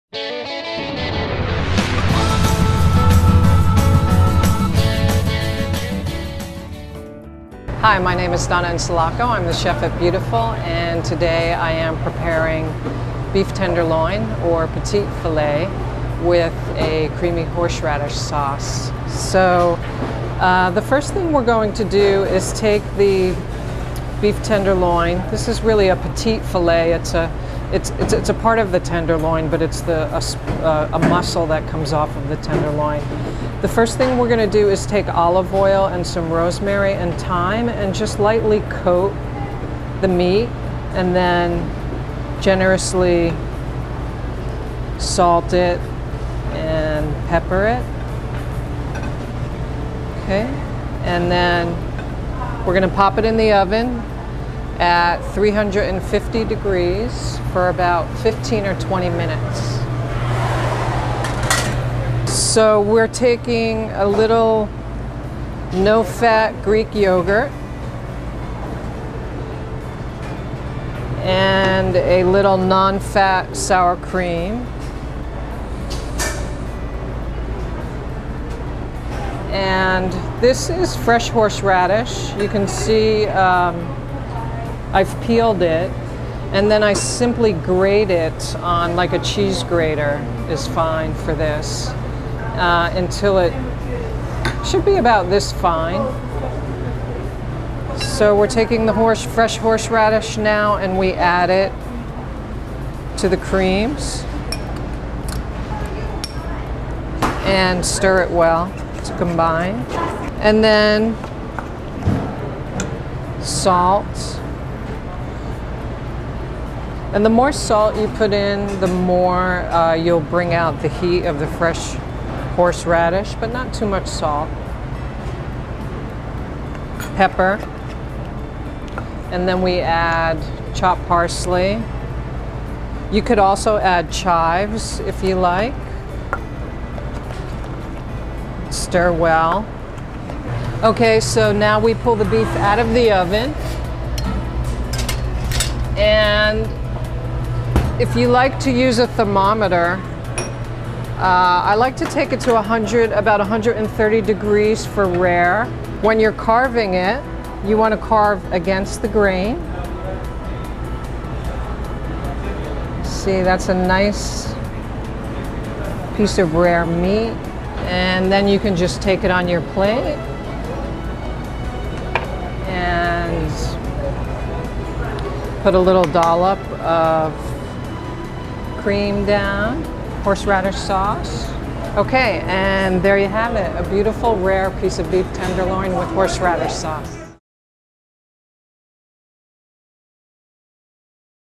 Roasted petite filet of beef with horseradish cream sauce
Recipe by Donna Insalaco of Beautifull Restaurant, San Francisco